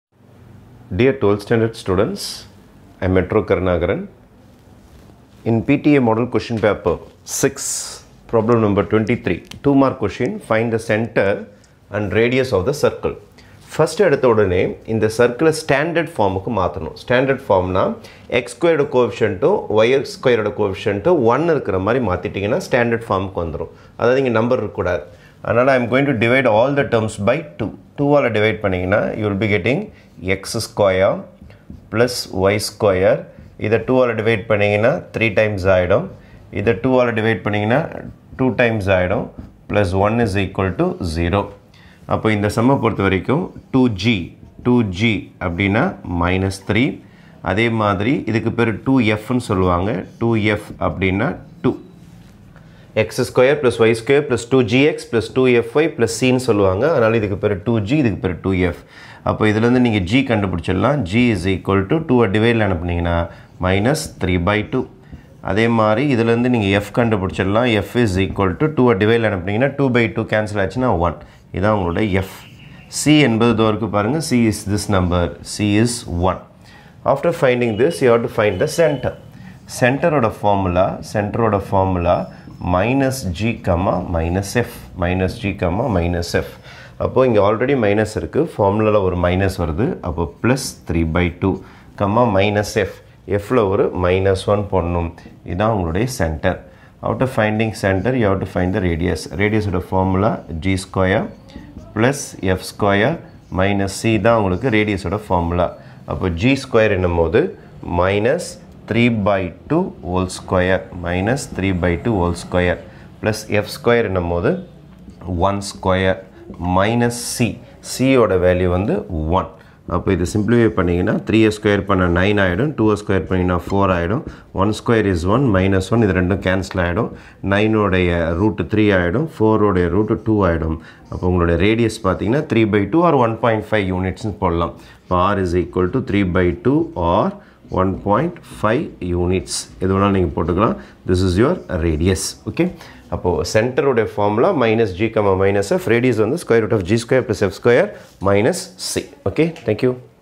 12th Std Maths PTA Model-6(23) Find centre and radius of the circle 2x^2+2y^2-6x+4y+2=0
12th Std MATHEMATICS

SOLVED PARENT TEACHER'S ASSOCIATION
MODEL QUESTION PAPERS